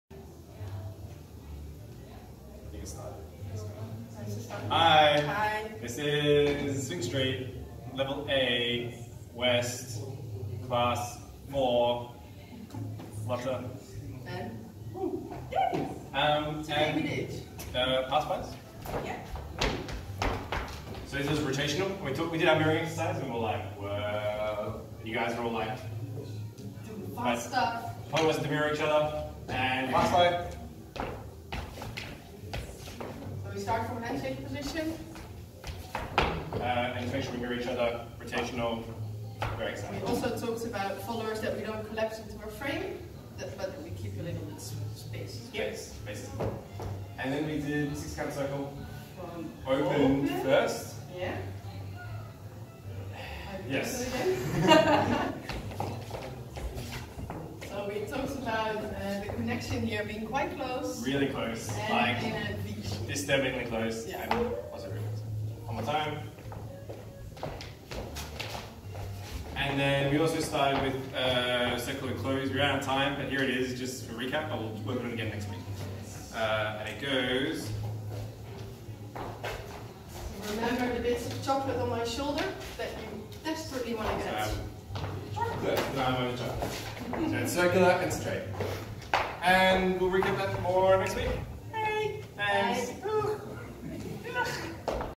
Beginners A, 4 - West - Lotte & Ben - Pass by and cicles
Class 4
- Mirror exercise
- Pass by
- 6 count circle from open
- 6 count circle from closed